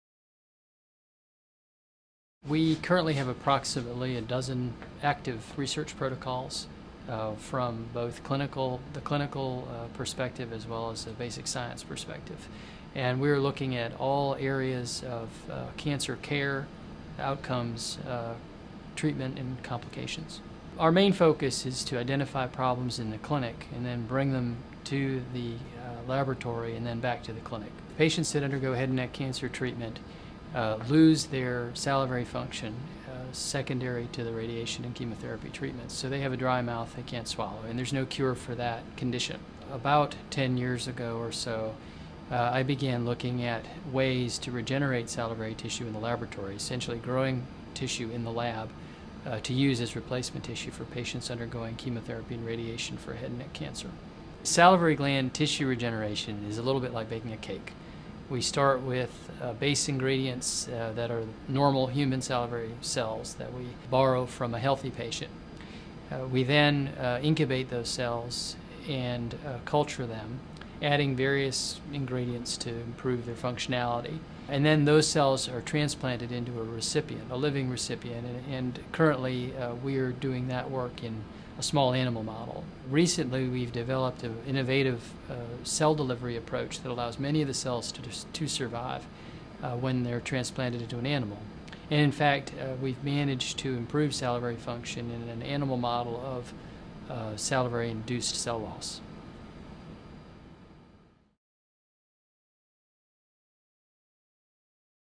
Tissue Engineering to Restore Salivary Function
Wake Forest Baptist Medical Center otolaryngologist Christopher Sullivan, MD, is using stem cell regenerative therapy to restore radiation-induced salivary gland dysfunction.